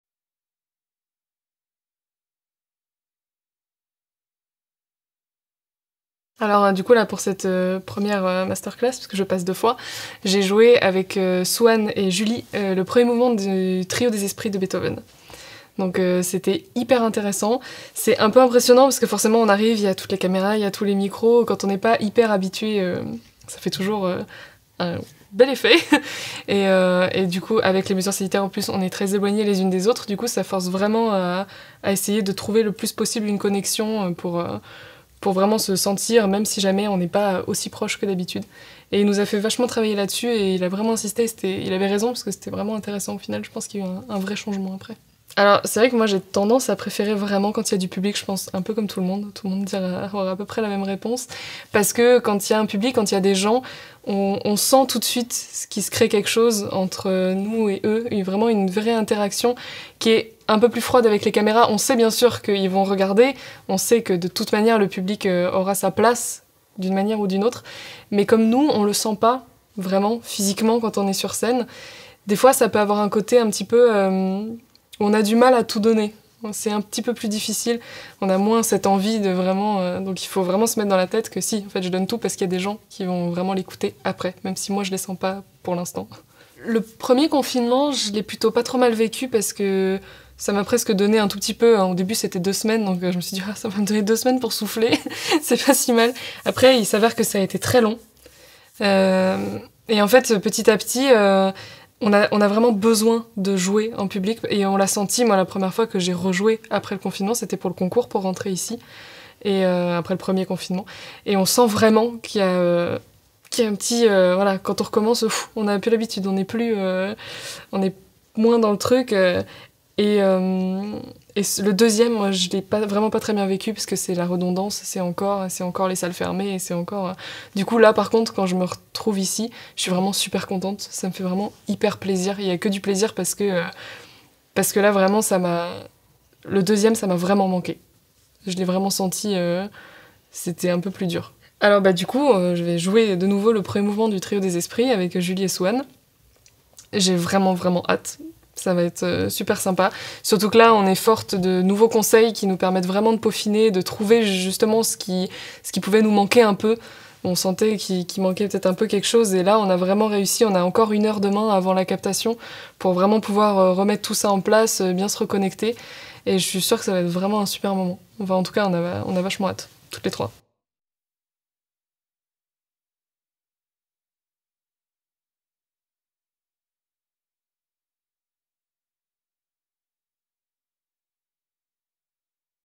Interview des Jeunes Talents : Justine Péré
Justine Péré partage ses impressions sur la masterclass de Christian-Pierre La Marca, exceptionnellement fermée au public pour raisons sanitaire.

Captation réalisée par notre partenaire SAE Institute Paris le 17 décembre 2020.